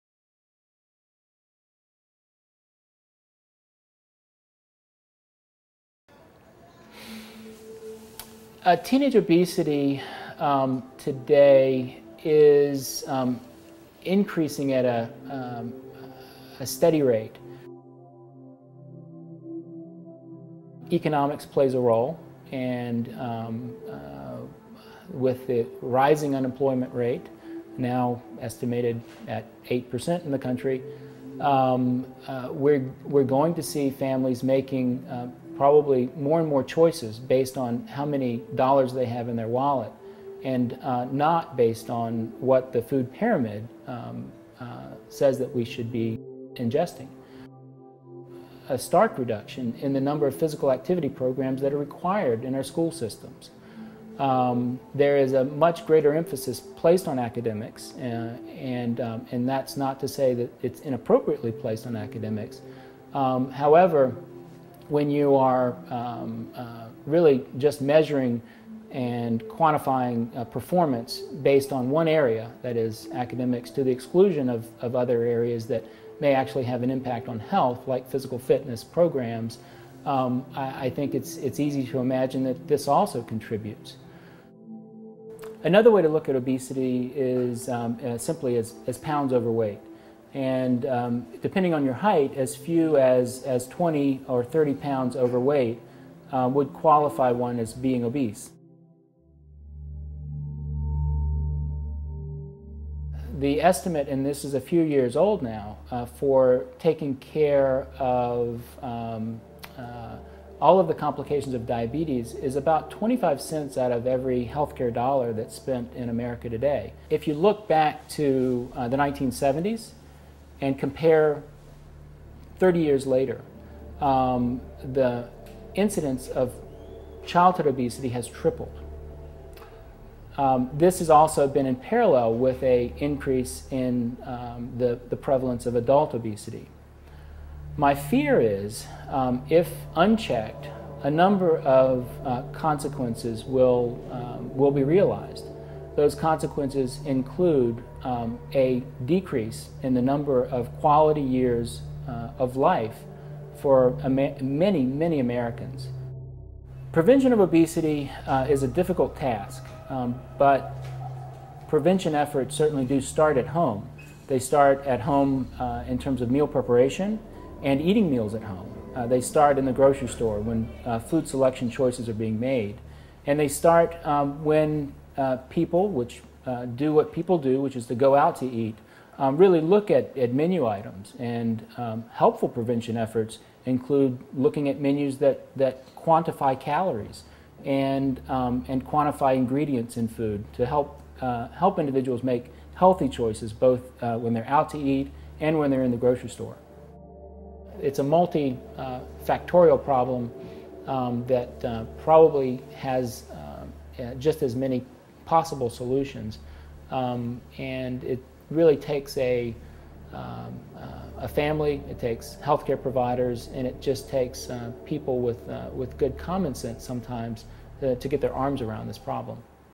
Dr. Thomas Inge on the implications of Childhood Obesity
Dr. Thomas Inge,  Surgical Director of the Cincinnati Childrens Surgical Weight Loss Program for Teens, discusses the long term implications of childhood obesity on one's health and America.